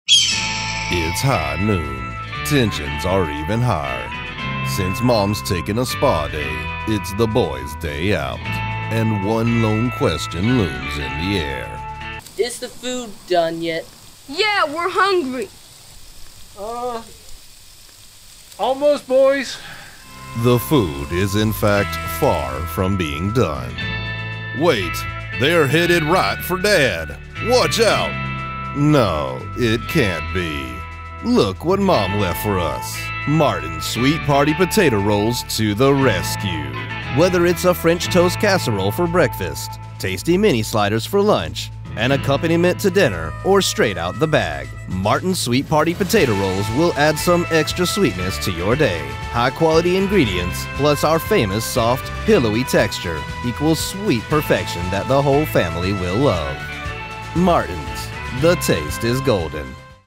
"The Showdown" - William D. (2nd Place)
A few months ago, we offered college students the opportunity to submit their best videos highlighting Martin's new Sweet Dinner and Sweet Party Potato Rolls, for the chance to win scholarship funds...And we were so pleased with the results! Congratulations to our 2nd place winner, William D, who created this video titled "The Showdown"!